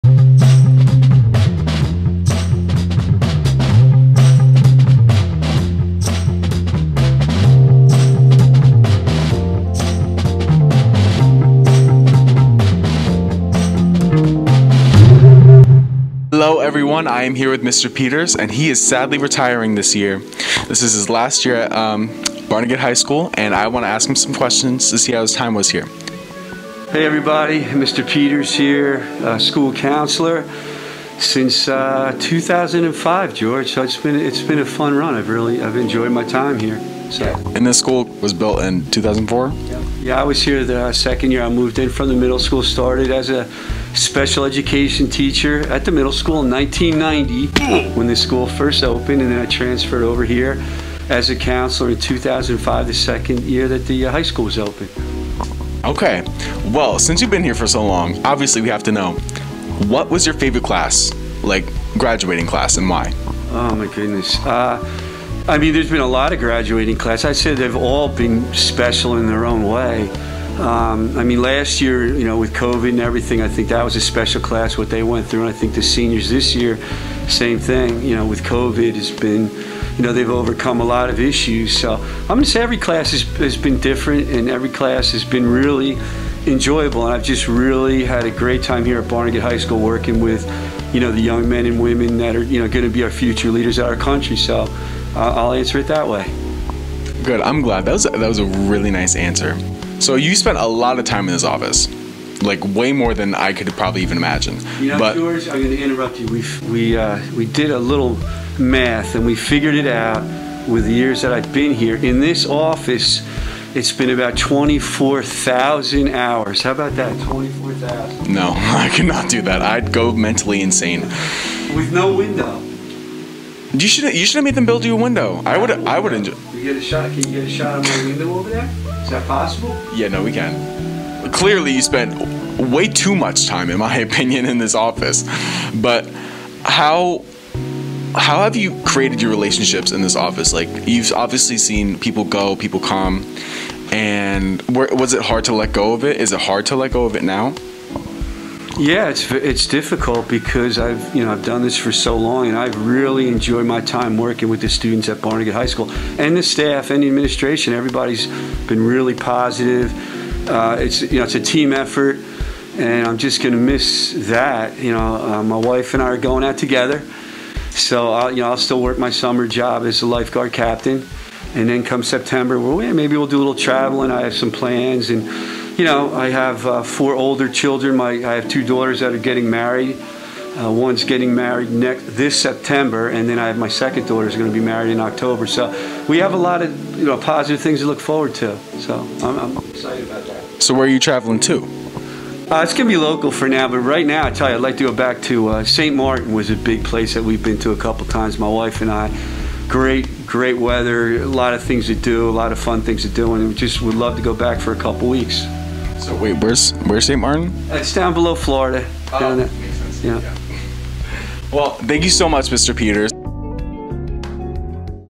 Mr. Peters is RETIRING?
George of George in Da HallsTM asks Mr. Peters to reflect on his time at Barnegat. Stop by and wish him well before the end of the year!